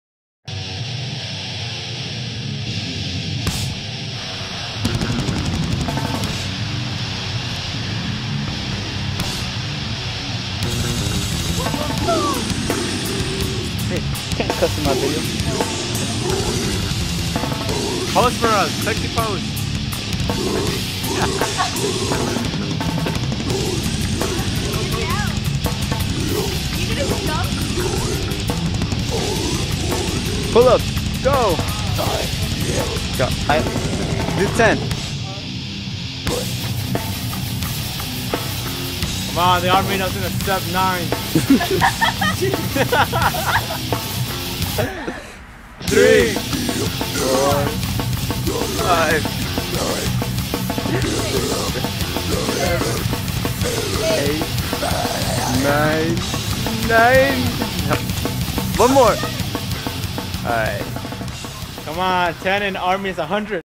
Nick
A friend getting on a roof and doing 10 pull ups after school.